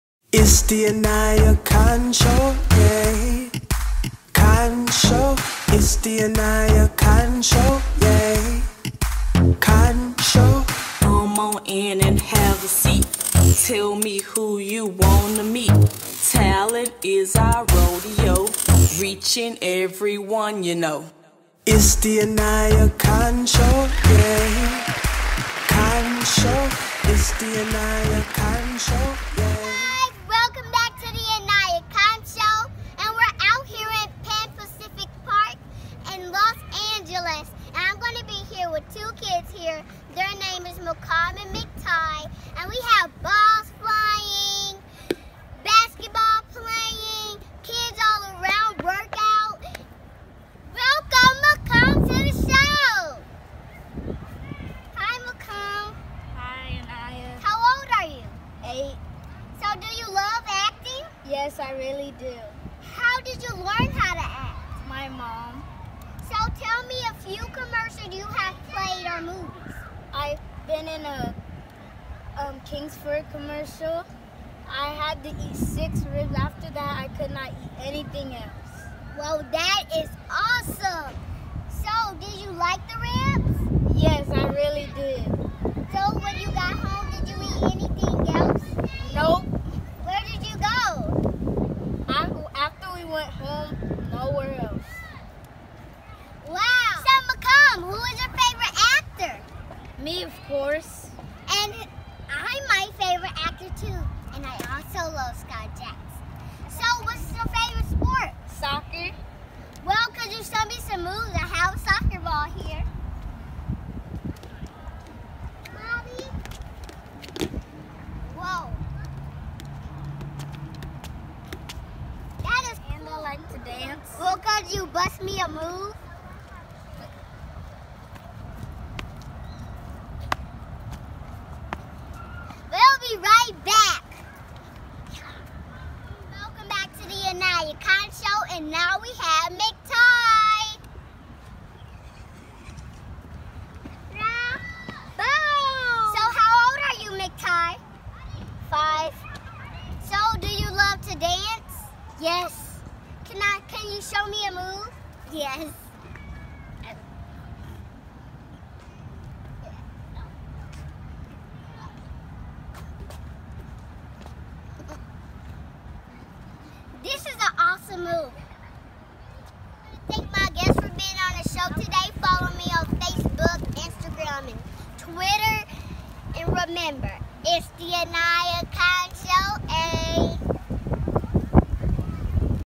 Aniyah In Pan Pacific Park LA Part 1
Aniyah takes on kids at Pan Pacific Park in LA for a small interview not only are we actors but we can also be backup dancers